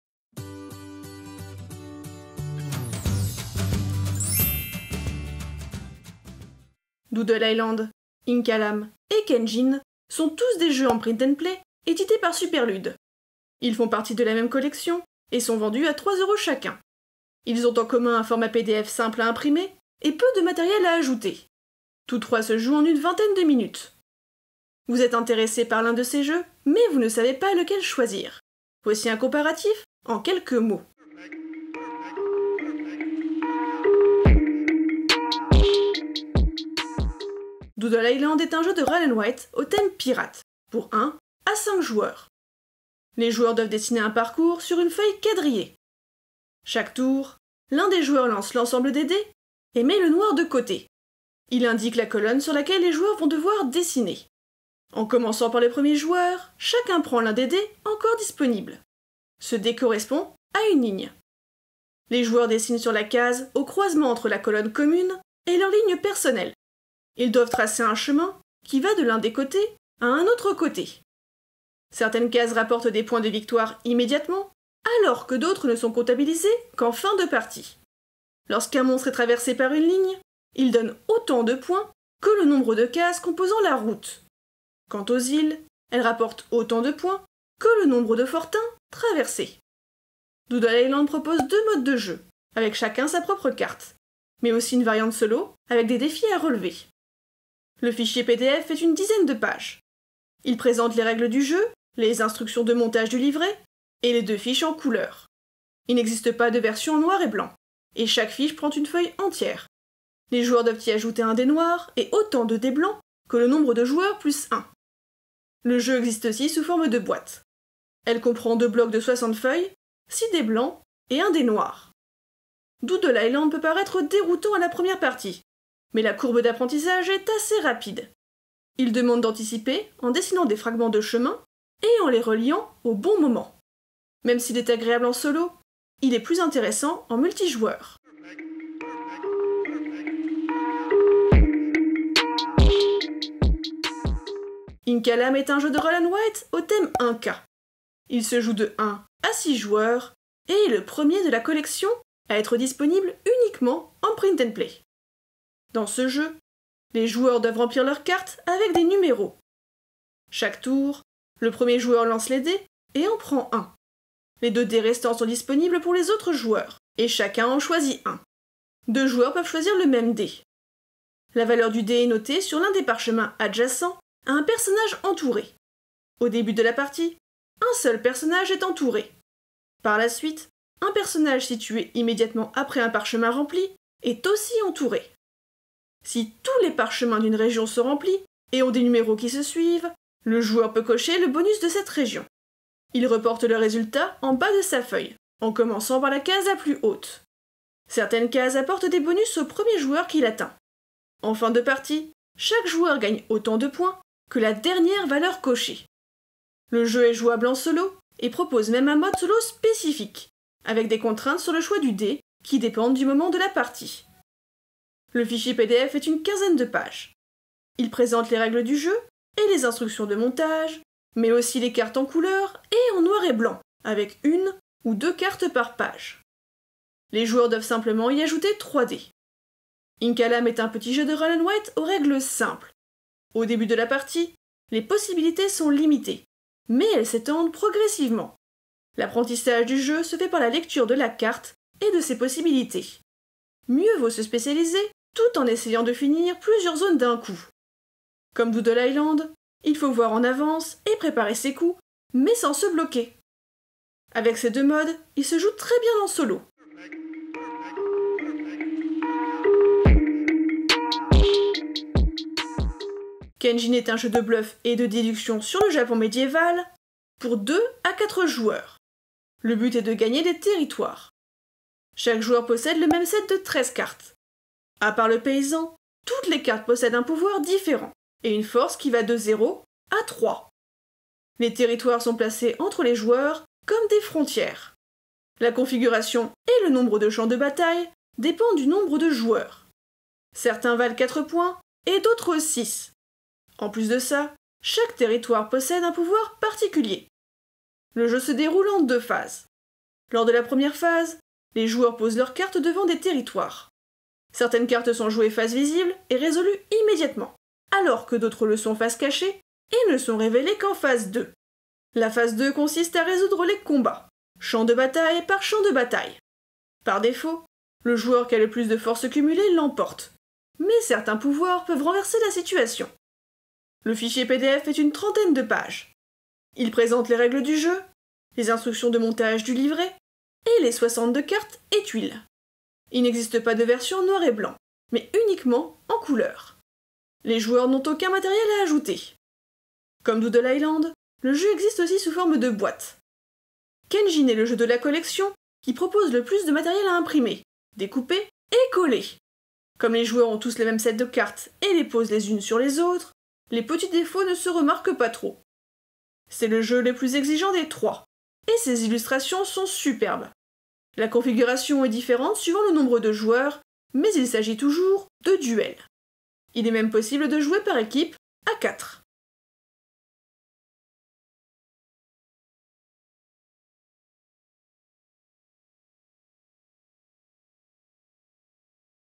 3 jeux en Print & Play
Doodle Islands, Inkalam, et Kenjin sont tous des jeux en Print&Play édités par Superlude. Ils font partie de la même collection et sont vendus à 3 € chacun. Ils ont en commun un format PDF simple à imprimer et peu de matériel à ajouter. Tous 3 se jouent en une 20ène de minutes.

Vous êtes intéressés par l’un de ces jeux, mais vous ne savez pas lequel choisir ? Voici un comparatif en quelques mots.